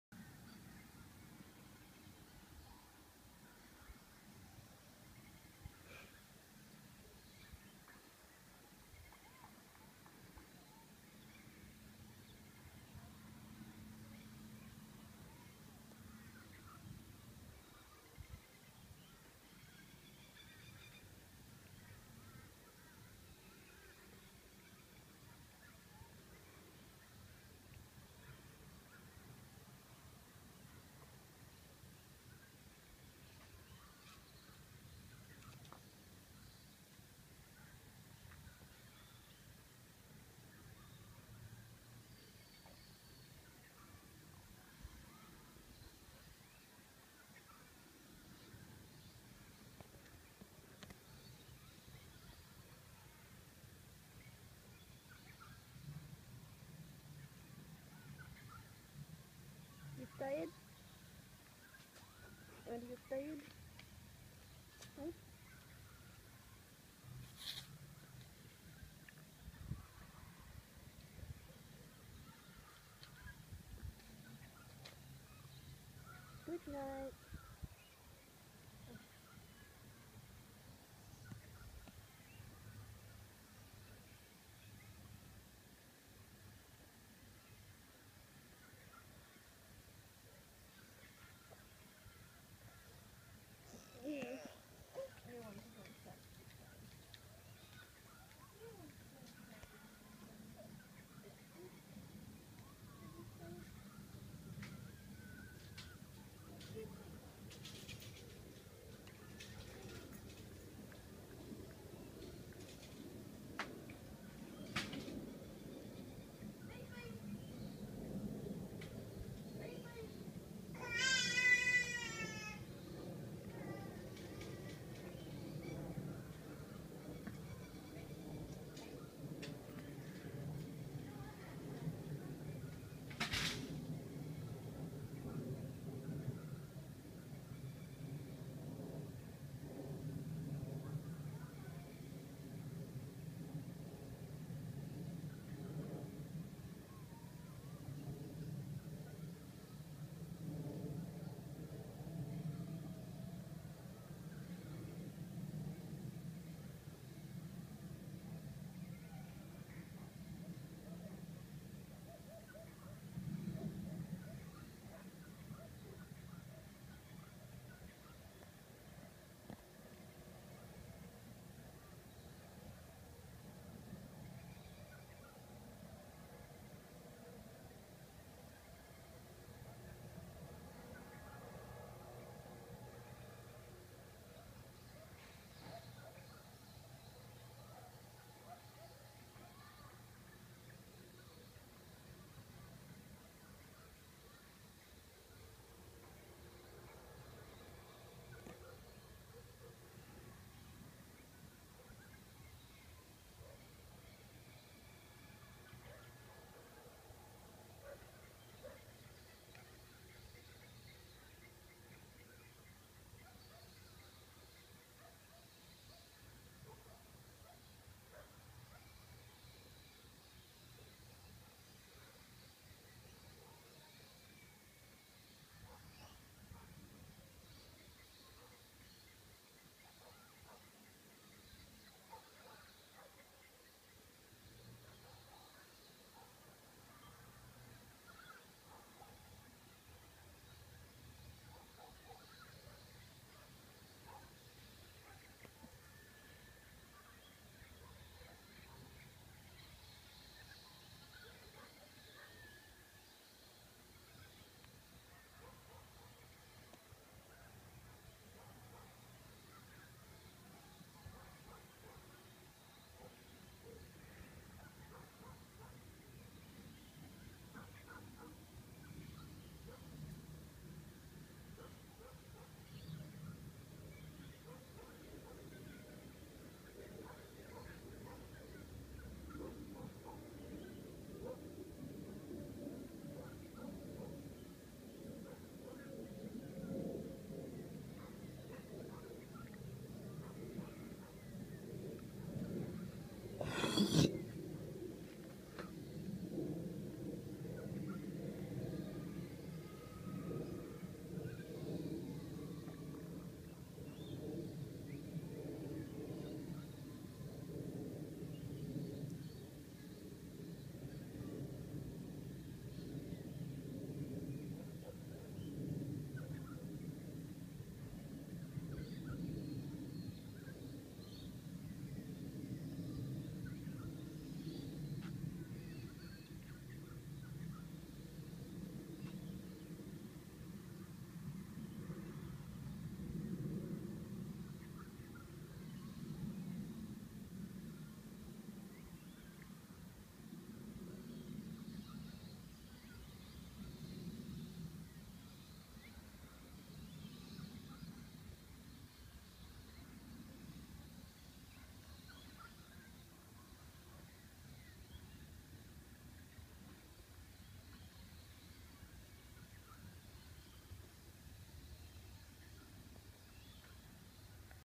Ladybugs mating
I legitimately filmed ladybugs having sex for 6 minutes